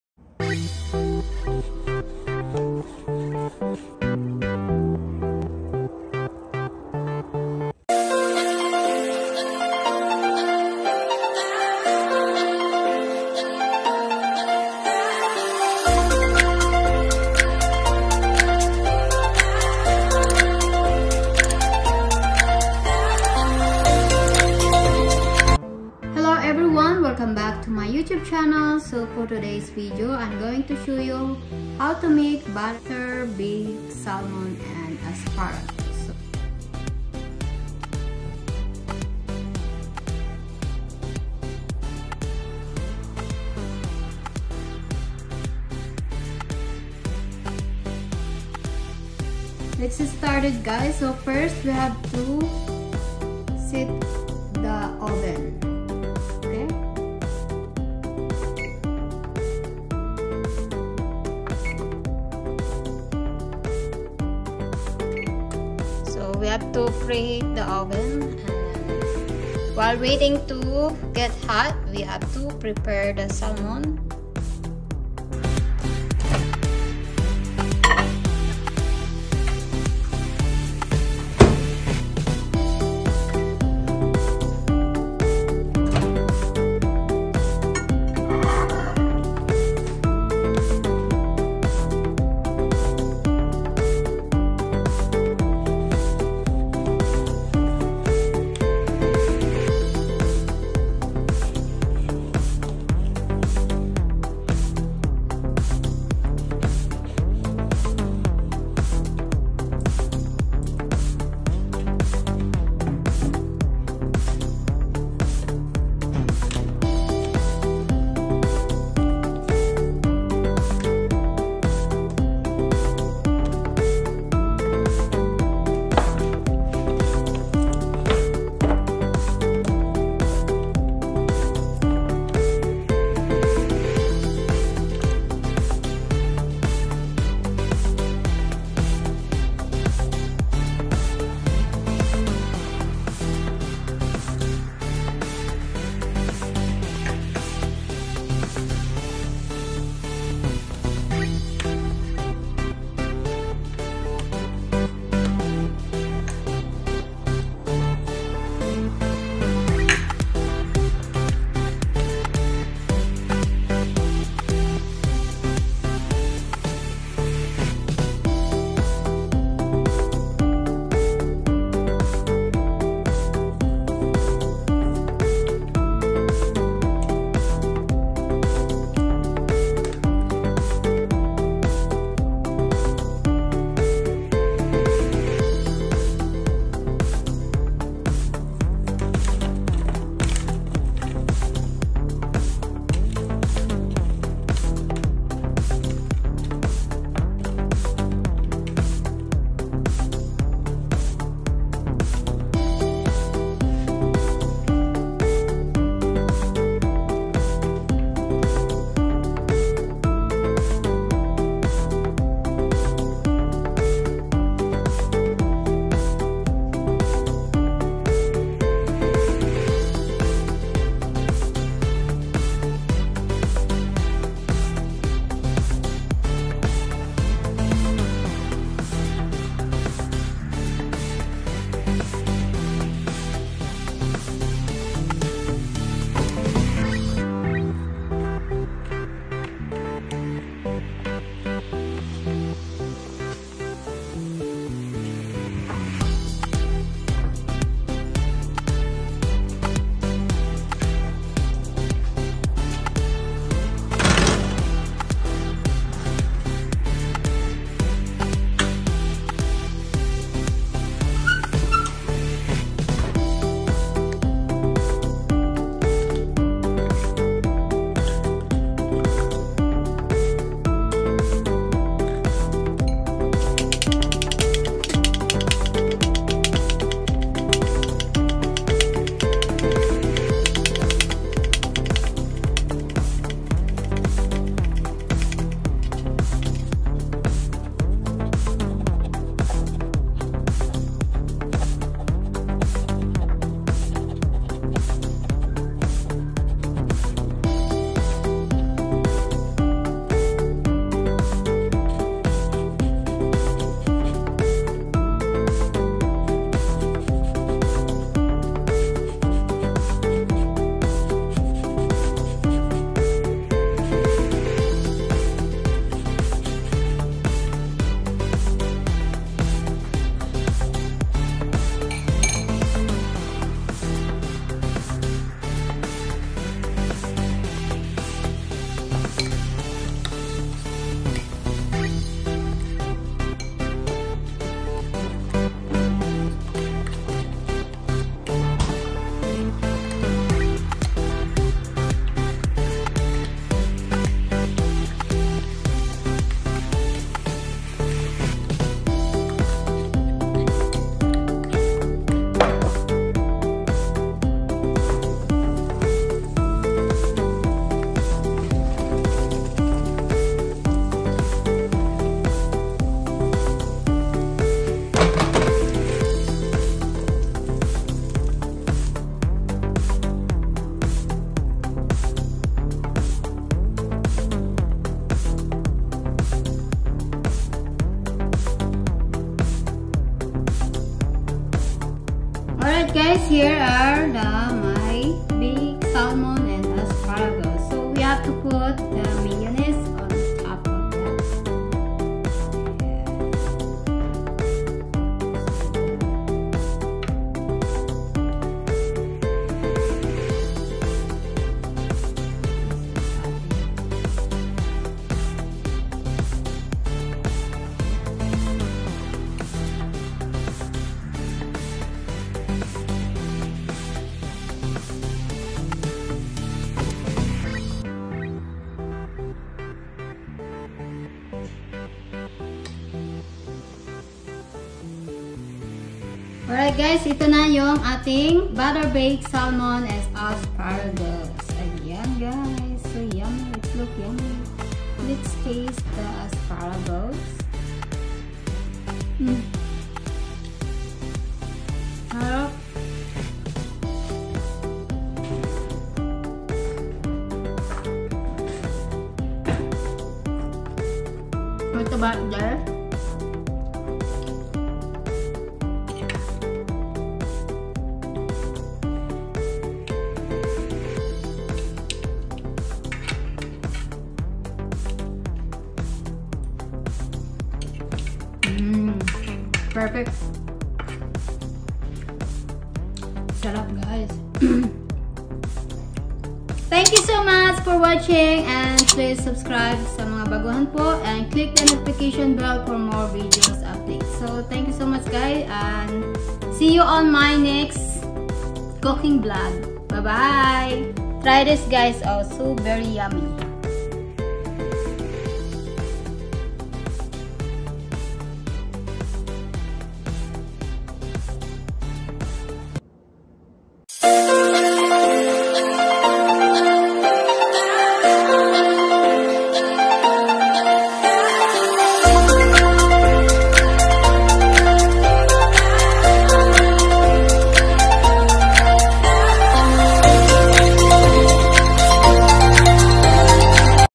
How to Make Butter Bake Salmon and Asparagus | Lysa Long
Butter Bake Salmon and Asparagus

Ingredients
3 tbsp butter
1 lb fresh salmon fillet’s
1 bunch fresh asparagus spears, trimmed
1/4 tsp salt
1/8 black pepper
1/3 cup mayonnaise
1 tbsp chopped green onion
2 tsp chopped fresh parsley
1/4 tsp finely shredded lemon peel
1 tsp lemon juice

Instructions
1. Preheat oven to 350 degrees Fahrenheit. In a small bowl melt the butter in microwave.
2. Place the Salmon skin side down, one side on a baking sheet. Add asparagus to the other side of the baking sheet. Drizzle melted butter over salmon and asparagus, sprinkle with salt and pepper. Bake 12 to 15 minutes or until salmon is cooked through and flakes easily with a fork and asparagus in tender.
3. In a small bowl combine mayonnaise, green onion, parsley, lemon peel and lemon juice.
4. Serve the Salmon and Asparagus on top with sauce.